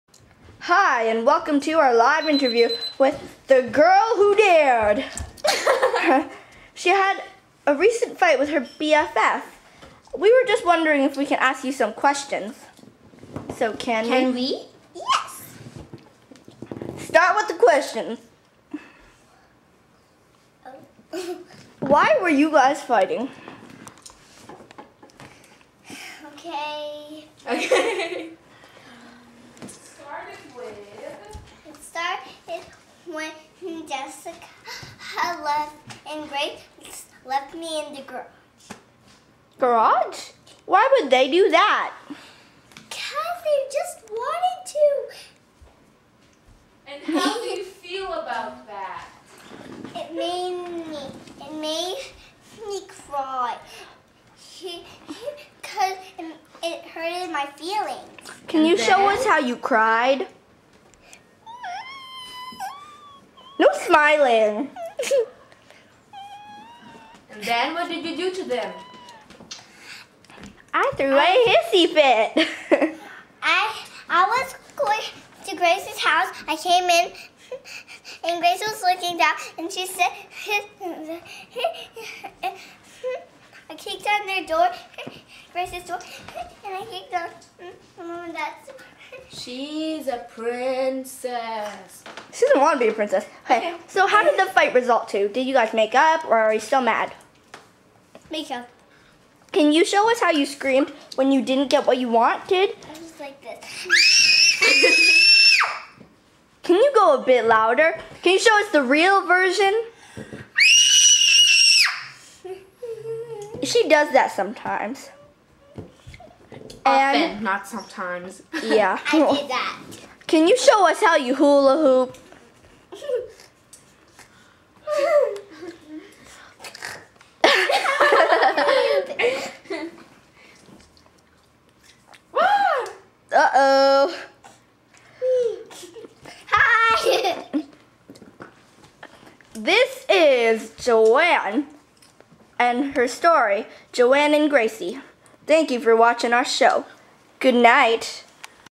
joanne's live interview
Interview of the girl who threw the hissy fit.